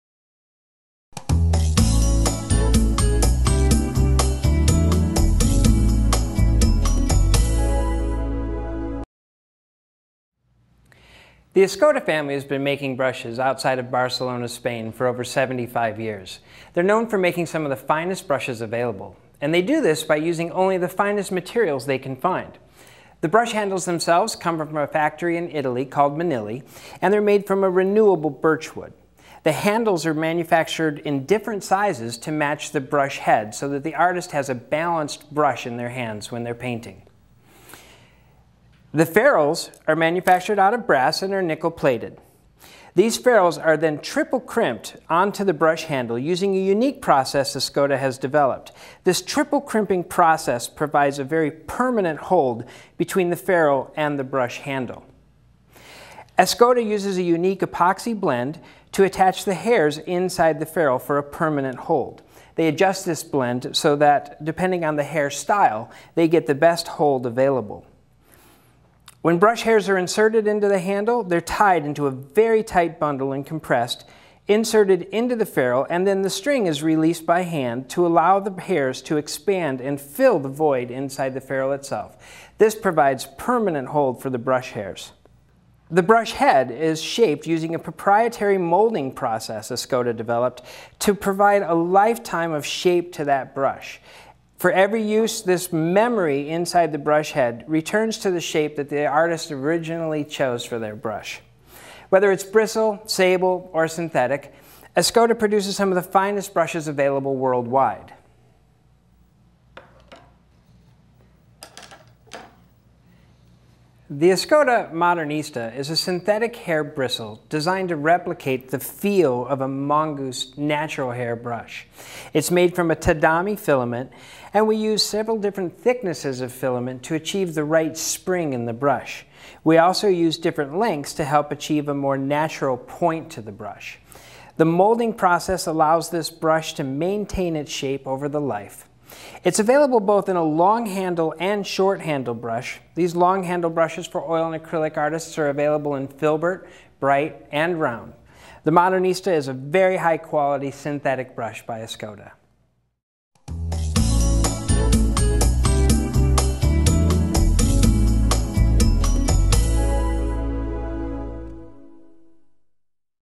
Escoda Tadami Synthetic Modernista Brushes
The Escoda family is known for crafting some of the finest brushes available. The Modernista is a synthetic bristle designed to replicate the feel of a Mongoose natural hair brush. See why these brushes are a great choice for acrylic and oil painters.